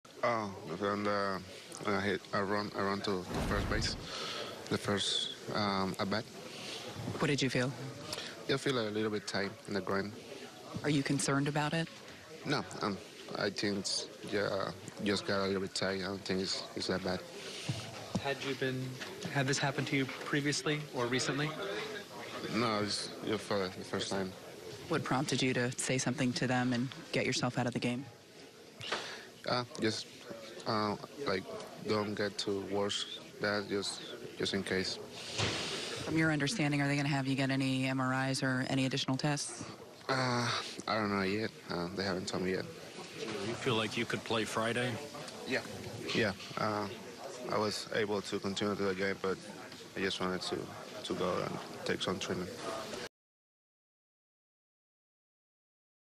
Gio Urshela feeling OK after leaving game with groin tightness
The Yankees appear to have dodged another injury bullet after Gio Urshela left Wednesday's game in the 5th inning due to groin tightness.